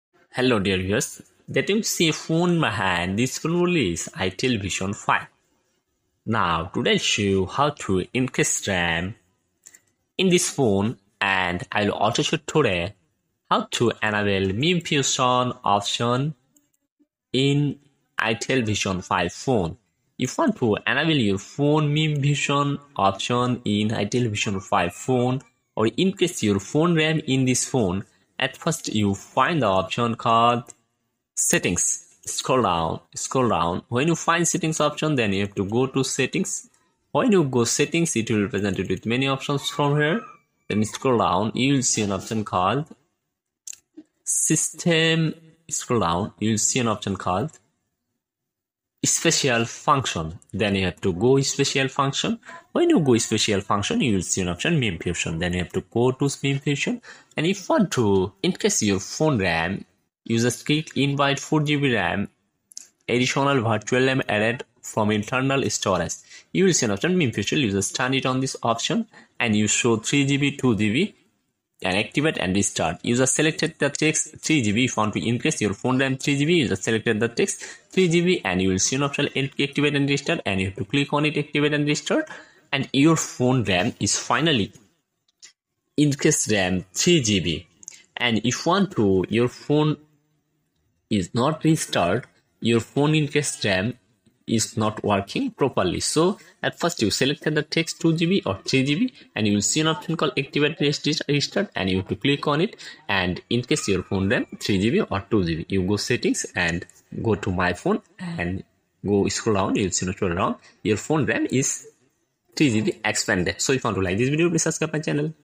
itel vision 5 increase Ram // How to enable MemFusion
This phone model is itel vision 5 phone. now today iw ill show you how to enable memfusion in itel vision 5 phone. and i will also show you today how to increase ram in itel vision 5 phone.